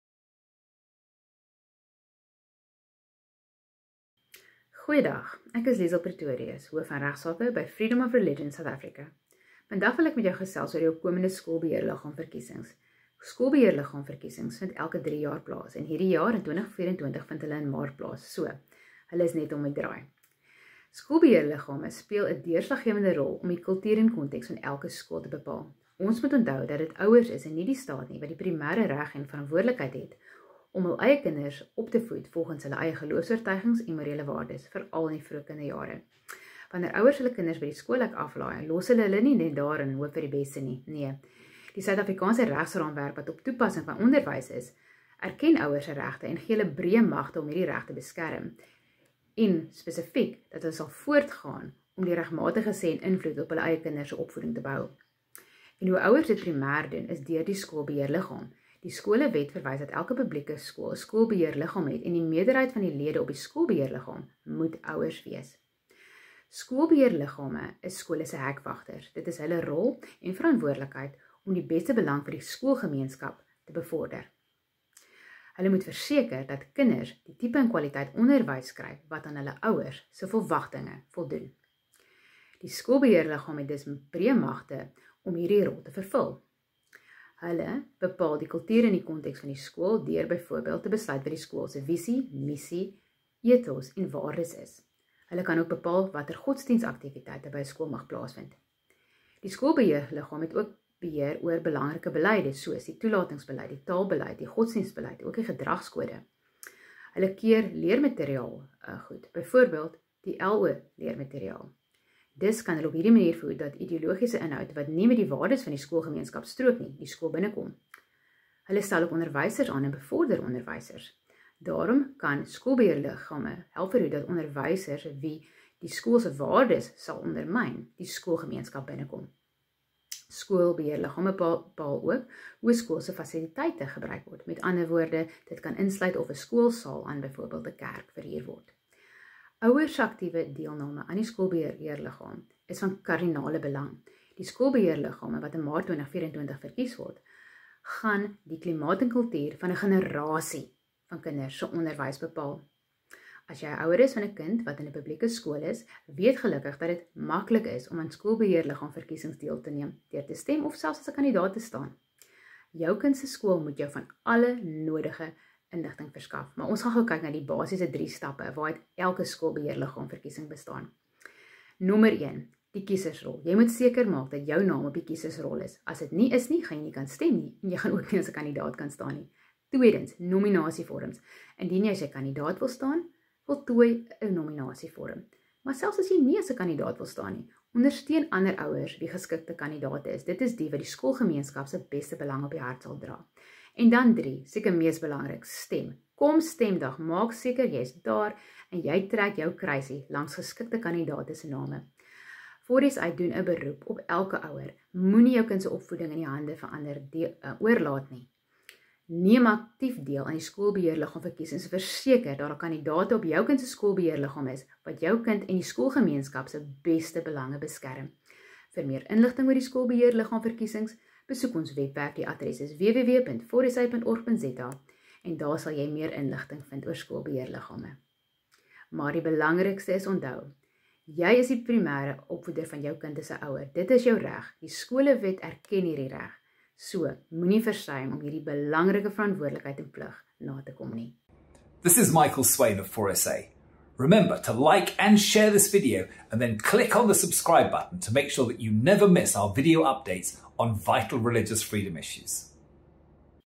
FOR SA - Skool Beheerliggaam Verkiesing - 20 Februarie 2024
Liesl Pretorius, hoof van Reg Sake by FOR SA gesels met jou vinning oor die opkomende Skool Beheerliggaam Verkiesing wat in Maart 2024 plaasvind, en alles wat jy moet weet.

vir meer inligting.